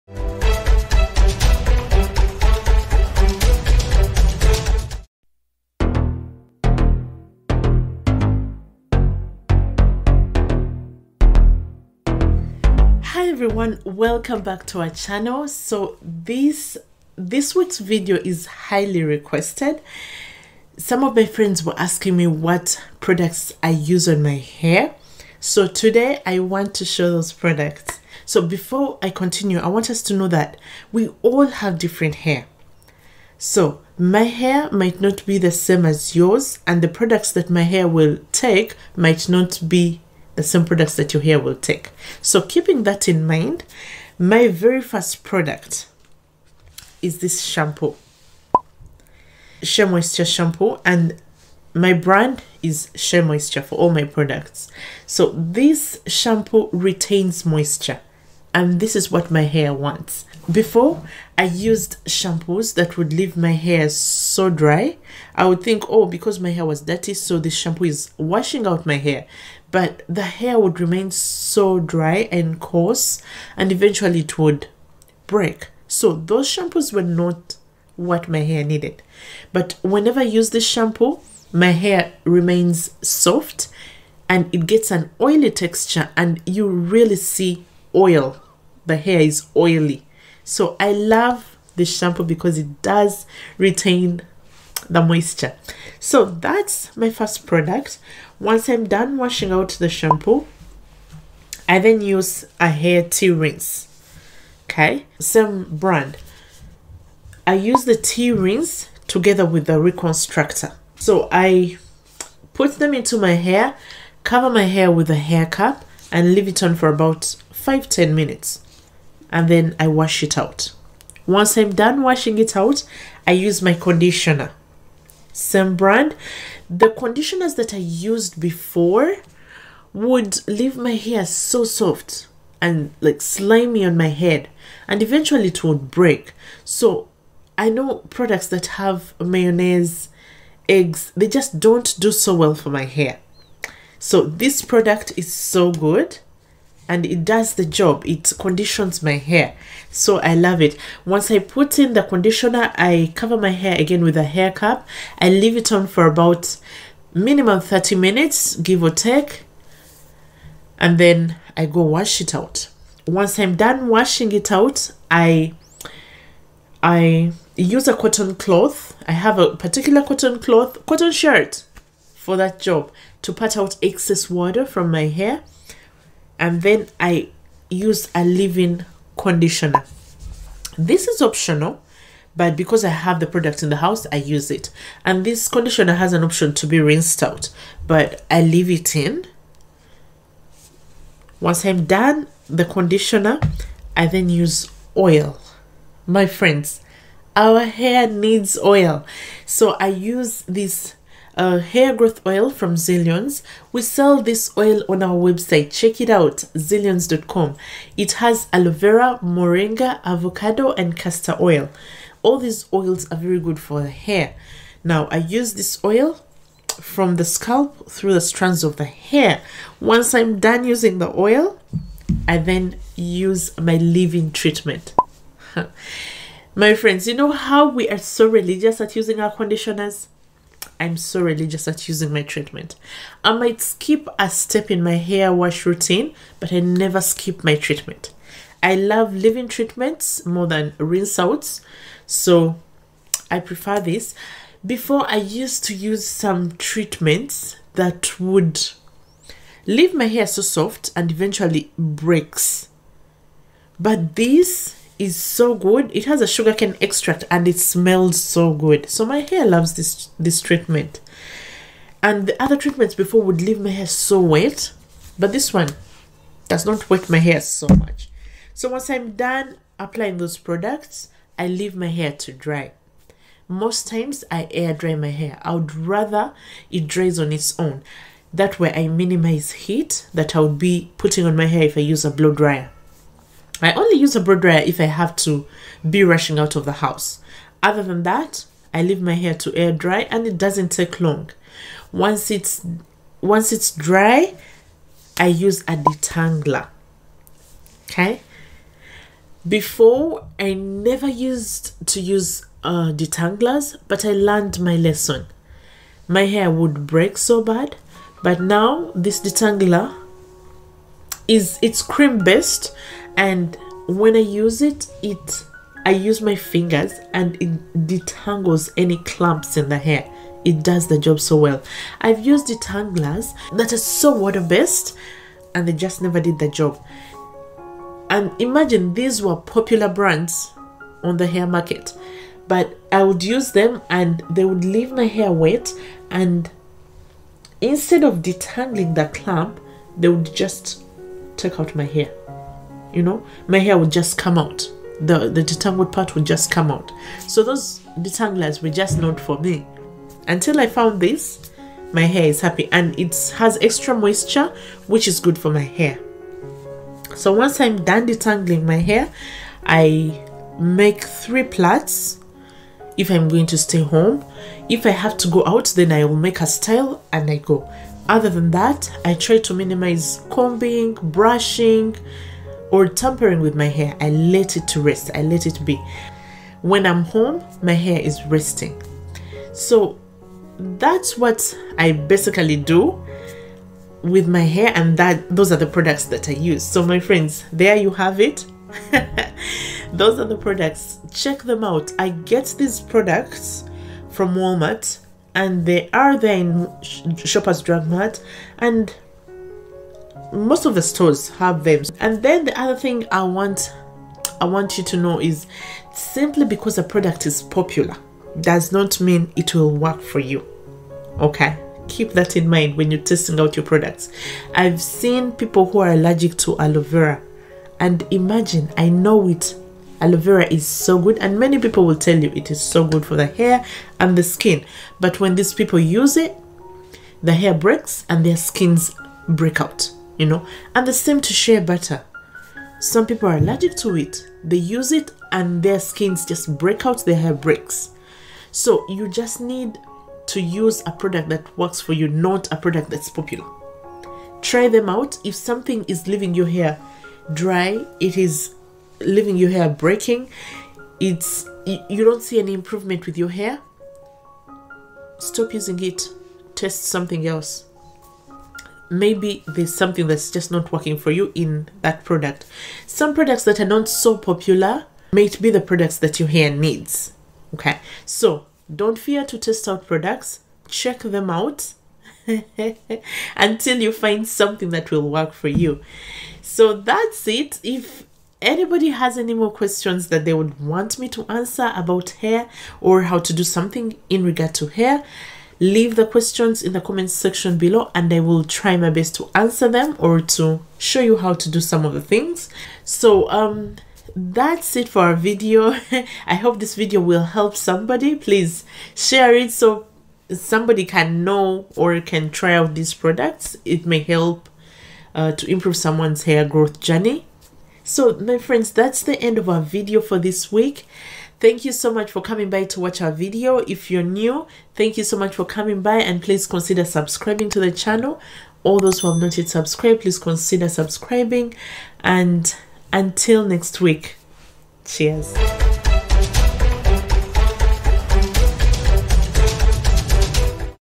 Steps on How I Care for My Hair and My Favorite Products
Hi friends,
In this video I am showing natural hair products that I use which  promote growth and healthy hair for me. I use Shea Moisture brand for my hair care products.

1. Zeelions hair growth oil can be bought from our

2. Shea moisture products can be found in most stores however I buy mine from Walmart.

Hair bundles and organic hair growth oil